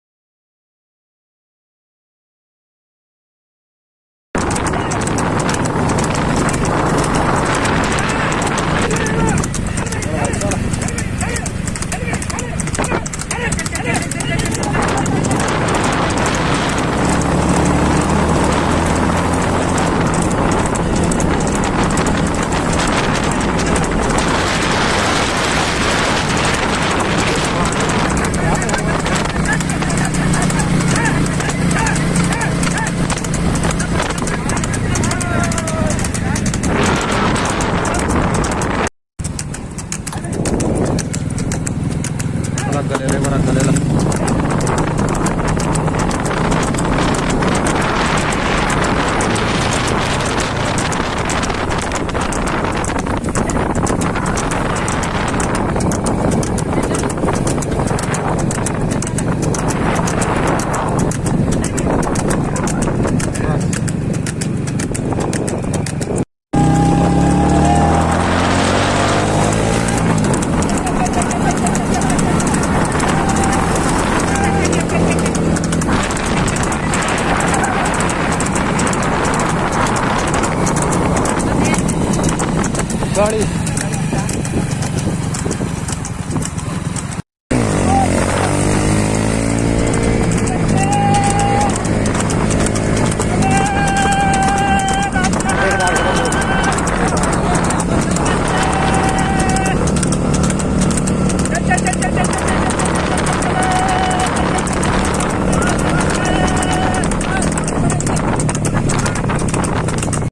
Rahul Gujjar !! मुढलाना !!District Haridwar ,jhota race full video बहुत कम मिलते हैं ऐसे लवारे 🐃🐃
2021,jhota race Ganga ji sey atey hovey, jhota race
hd, jhota race hd video, jhota buggi race, jhota
race song, horse, jhota race